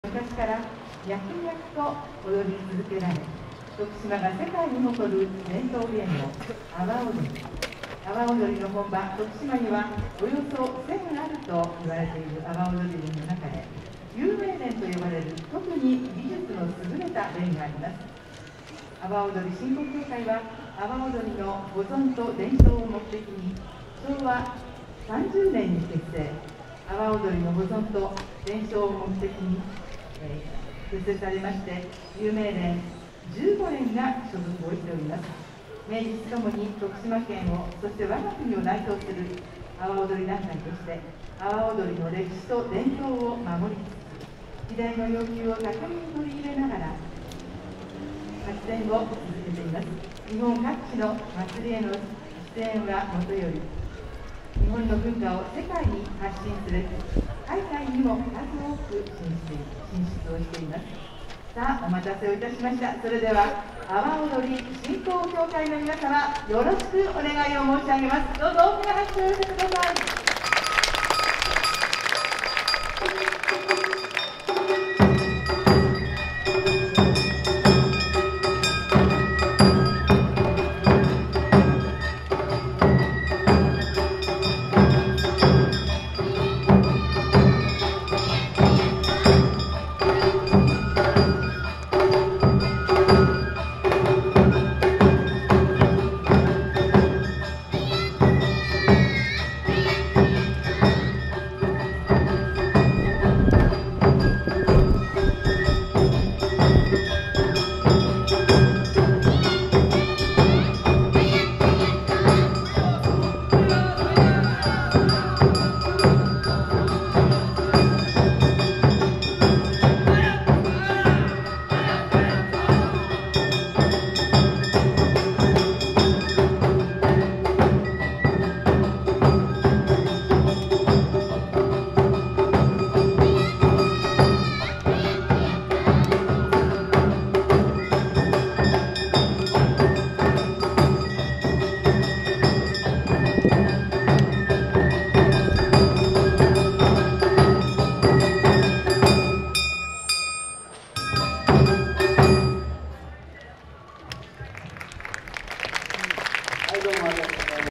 阿波おどり紹介【4K】／2回目／第9回成田伝統芸能まつり秋の陣
2022/09/17（令和4年9月17日）
第9回成田伝統芸能まつり秋の陣
会場：成田山総門前広場および成田山表参道
主催：成田伝統芸能まつり実行委員会
協賛：イオンモール成田、米屋株式会社、日本航空株式会社他